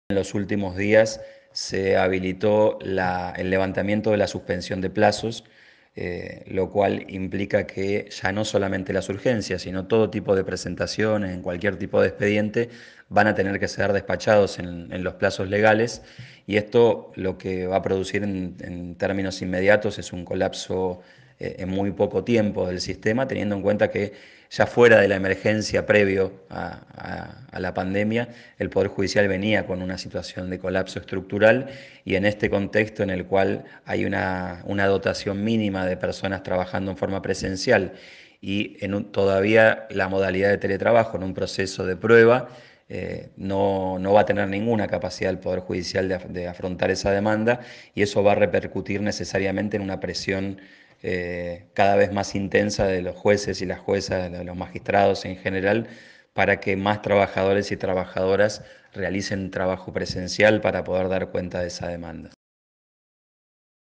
🎙️ Pablo Abramovich, Sec. Gral de la AJB, sobre el levantamiento de la suspensión de términos
🎙️ Pablo Abramovich, Sec. Gral de la AJB habla sobre el levantamiento de la suspensión de términos para FM La Tribu.

(Audio 1 de 2) 🎧